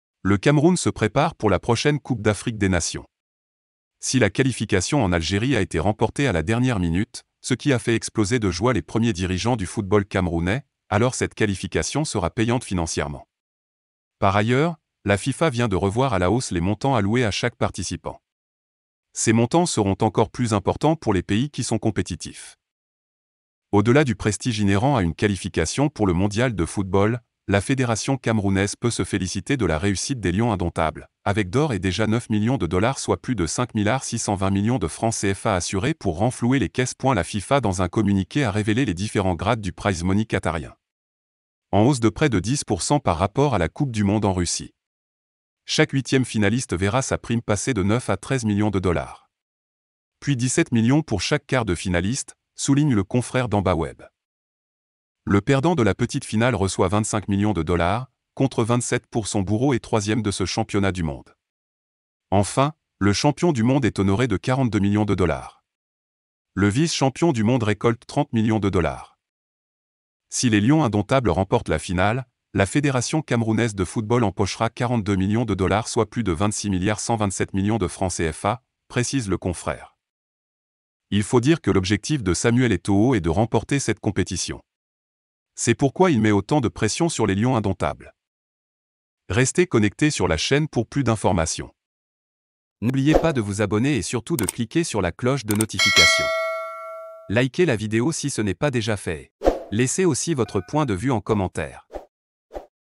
Mondial 2022: Samuel Eto'o frappe un grand coup
Le Cameroun se prépare pour la prochaine Coupe d’Afrique des nations. Si la qualification a été acquise dans les dernières minutes en Algérie, ce qui a fait davantage exploser de joie les premiers responsables du football camerounais, c’est ce que cette qualification va rapporter financièrement. D’ailleurs, la FIFA vient de revoir à la hausse les montants à allouer à chaque participant. Ces montants seront encore plus conséquents pour les pays qui iront loin dans la compétition.
« Au-delà du prestige inhérent à une qualification pour le Mondial de football, la fédération camerounaise peut se féliciter de la réussite des lions indomptables, avec d’ores et déjà 9 millions de dollars (soient plus de 5millards 620millions de FCFA) assurés pour renflouer les caisses.La FIFA dans un communiqué a révélé les différents grades du prize money qatarien. En hausse de près de 10% par rapport à la Coupe du monde en Russie. Chaque huitième finaliste verra sa prime passer de 9 à 13 millions de dollars. Puis 17 millions pour chaque quart de finaliste », souligne le confrère Ndamba Web